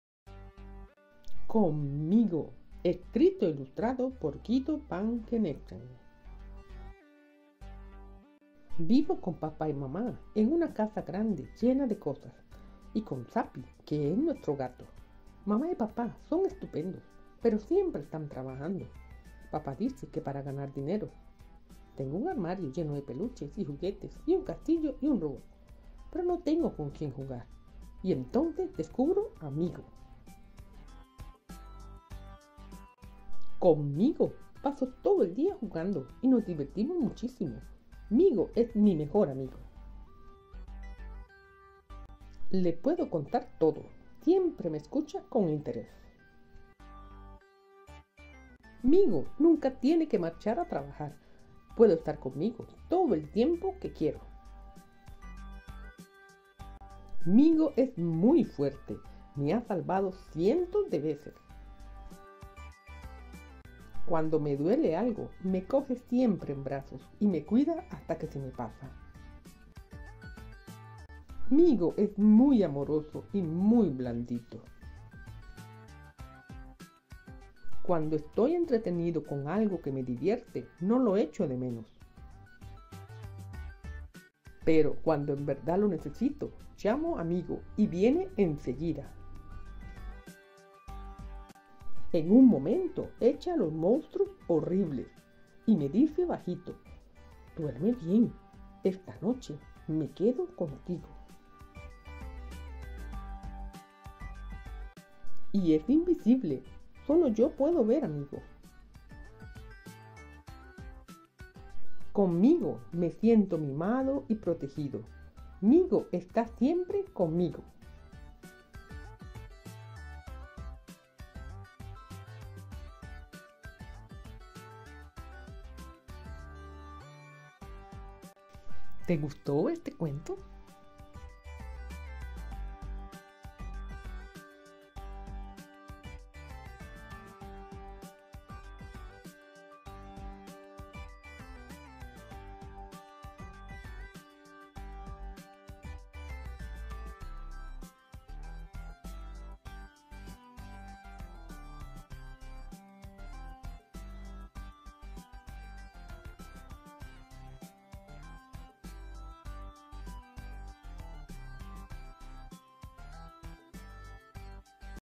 CON MIGO VIDEOCUENTO
Un niño solitario cuenta sus aventuras con un amigo imaginario llamado Migo y cómo disfruta de su dulce compañía.